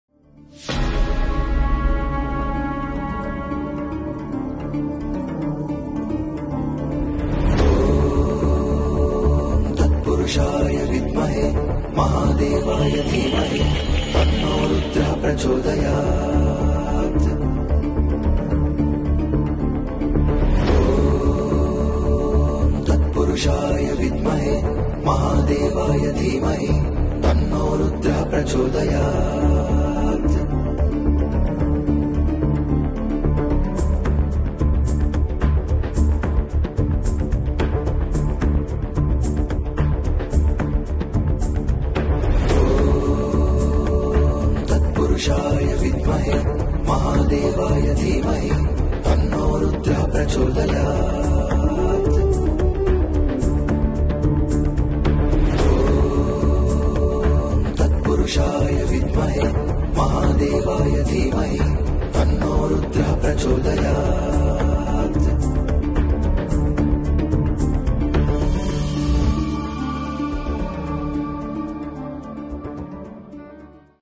Blessing New Beginnings: Bhumi Pujan at KWT Pimpri
With blessings, tradition, and a shared sense of purpose, we laid the foundation for KWT Phase 2—a space designed to shape the future of business and innovation. The Bhumi Pujan ceremony marked an auspicious beginning, celebrating the start of new opportunities, growth, and prosperity!